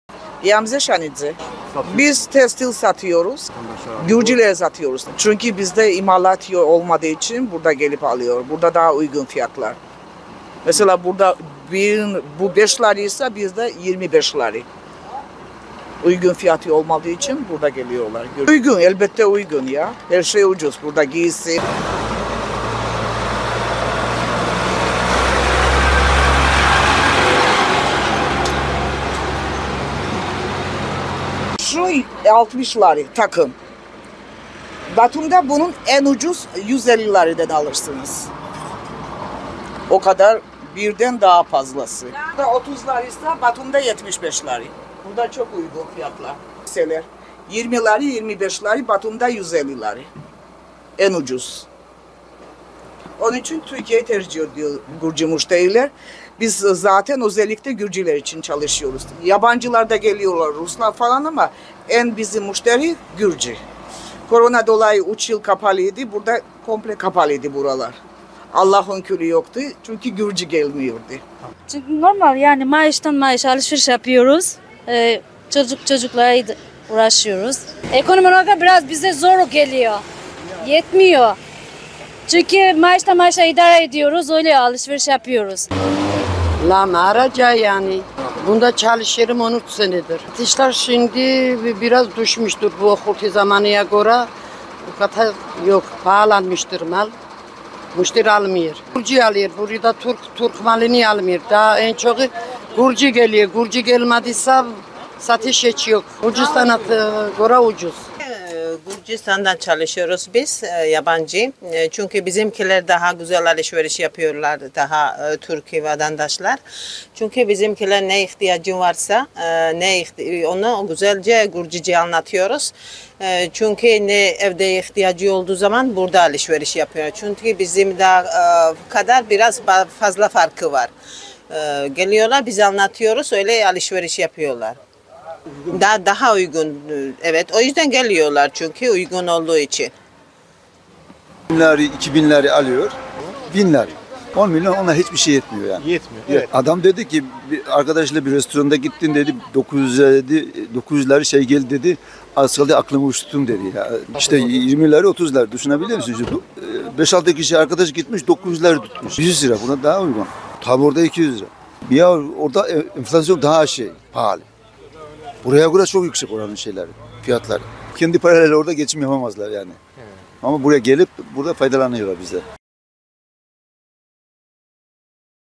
Gürcüler gelmez ise esnafın cebine para girmiyor! Hem çalışıyor hem de alışveriş yapıyorlar...
Artvin’in Kemalpaşa ilçesine gelen Gürcü vatandaşlar hem çalışıyor hem de ucuz buldukları için bolca alışveriş yapıyor. Kemalpaşa’ya yerleşerek tekstil mağazası açan İamze Shanidze, Gürcistan’da imalat olmadığı için özellikle kıyafetler çok pahalı mesela bu takım Türkiye’de 60 lari ama Batum'da ise 150 lari ve o yüzden de Gürcüler için Kemalpaşa’da her şey ucuz ve burada Gürcüler yoksa alışveriş de olmaz" dedi.